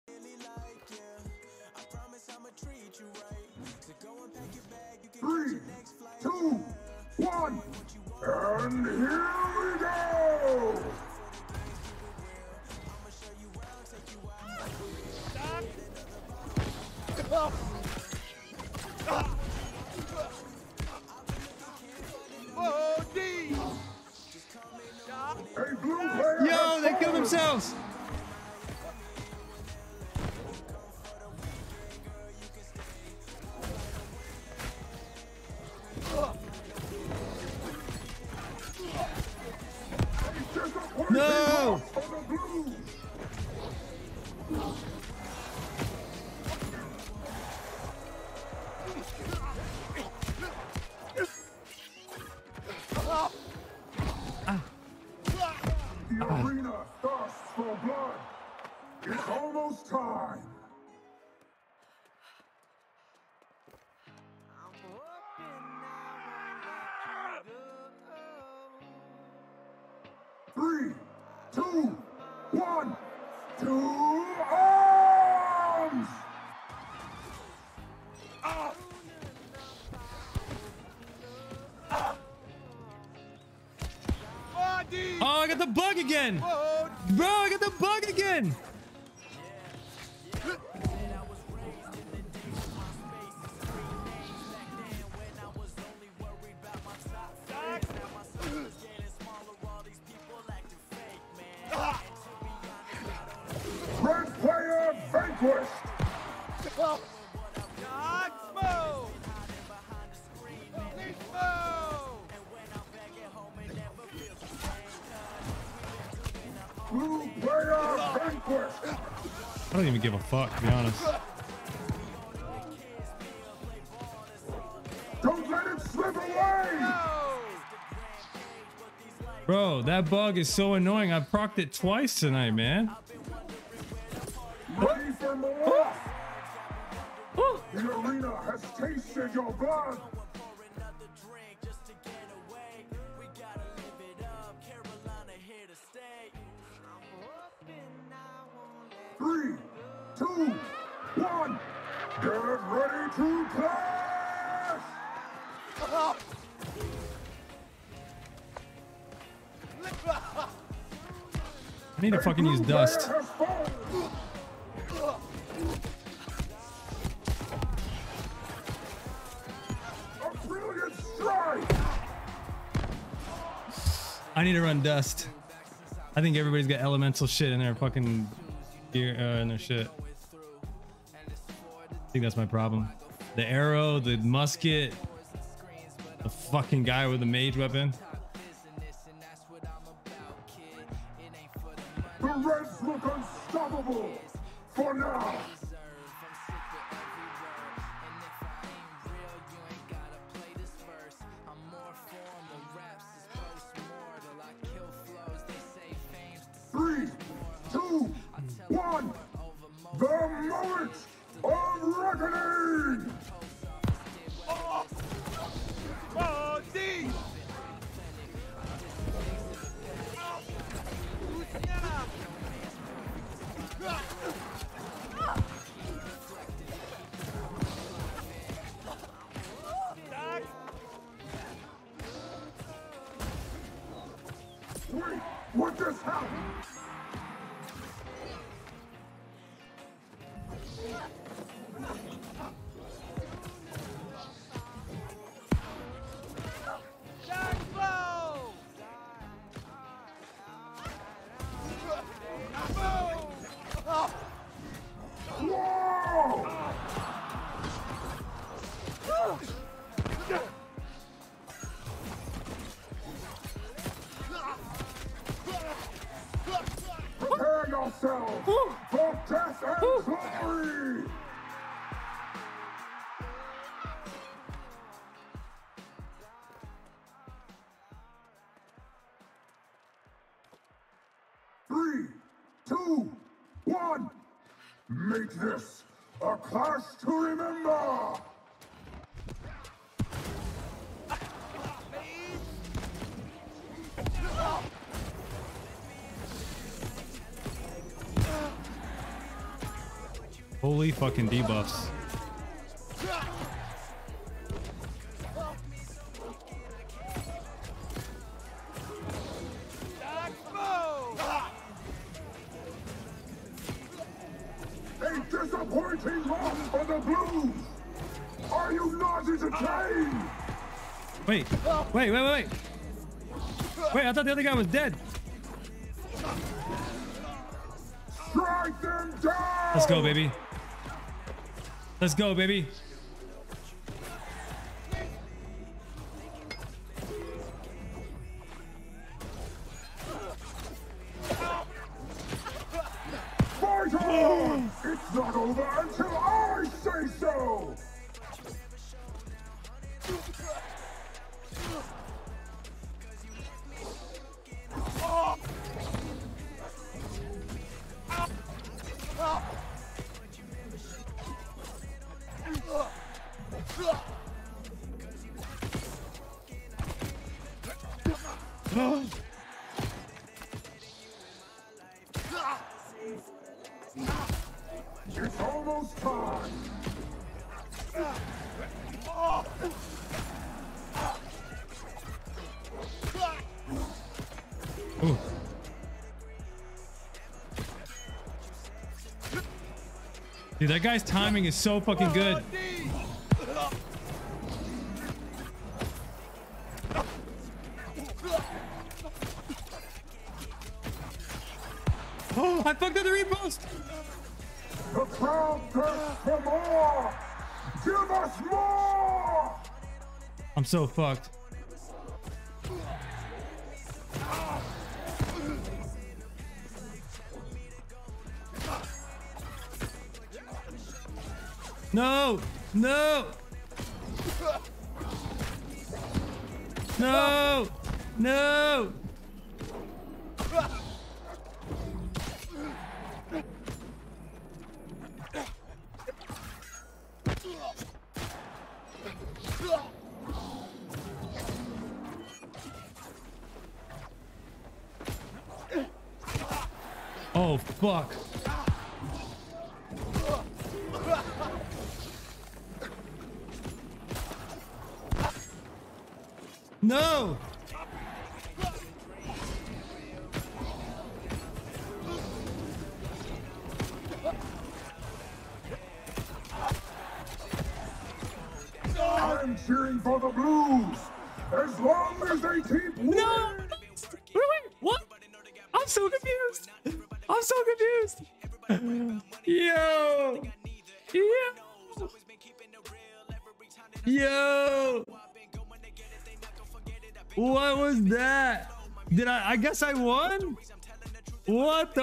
The craziest 3v3 win and you have to watch the last round BIGTESTYHEALS
The absolute craziest win that I clutched solo all the way to the very end!